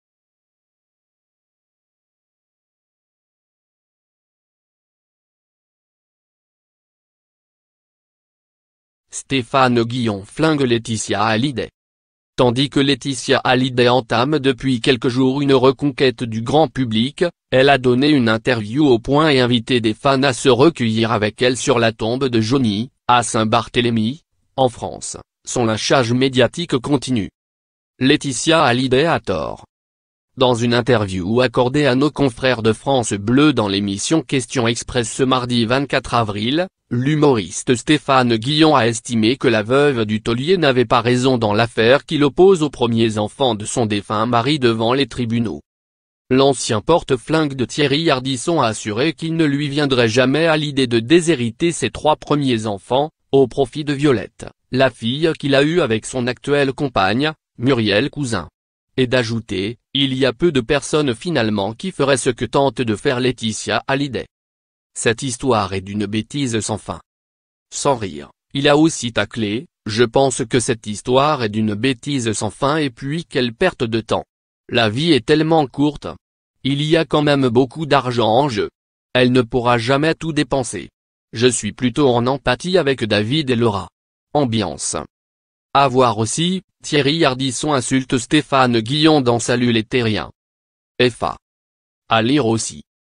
Stéphane Guillon flingue Laeticia Hallyday
Tandis que Laeticia Hallyday entame depuis quelques jours une reconquête du grand public - elle a donné une interview au Point et invité des fans à se recueillir avec elle sur la tombe de Johnny, à Saint-Barthélémy - en France, son lynchage médiatique continue...

Laeticia Hallyday a tort

Dans une inter­view accor­dée à nos confrères de France Bleu dans l'émis­sion Ques­tions Express ce mardi 24 avril, l'humo­riste Stéphane Guillon a estimé que la veuve du Taulier n'avait pas raison dans l'affaire qui l'oppose aux premiers enfants de son défunt mari devant les tribunaux.